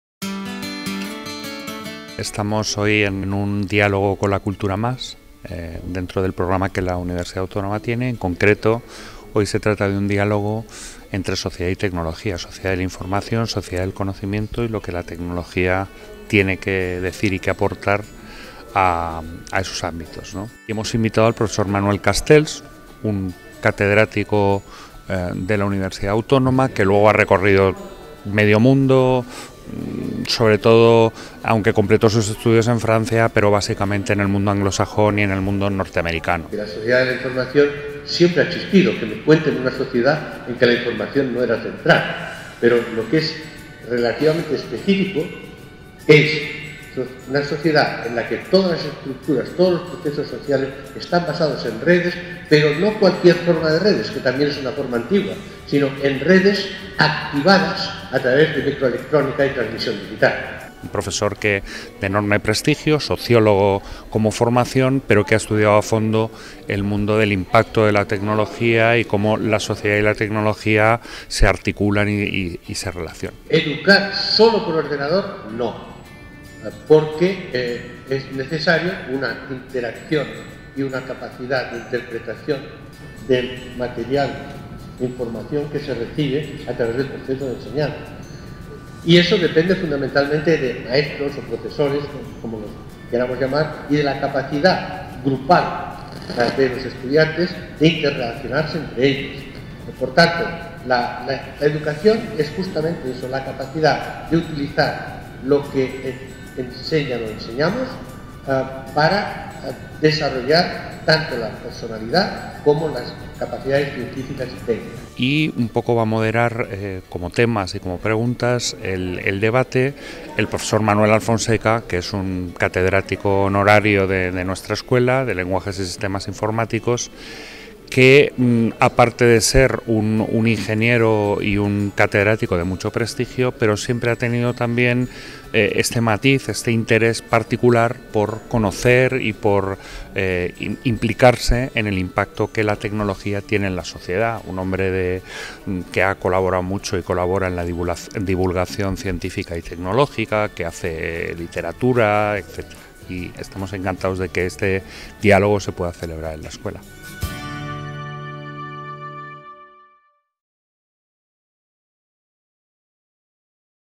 En "Diálogos con la Cultura": Manuel Castells Oliván
Manuel Castells Oliván, catedrático emérito de Sociología de la Universidad de California en Berkeley y Académico de la Real Academia de las Ciencias Económicas y Financieras pronunció la conferencia "La interacción entre TIC y la sociedad de la información".
La presentación de este acto corrió a cargo de Manuel Alfonseca, catedrático y profesor honorario de la Escuela Politécnica Superior.

El evento tuvo lugar en el Salón de Actos de la Escuela Politécnica Superior el día 31/03/2016.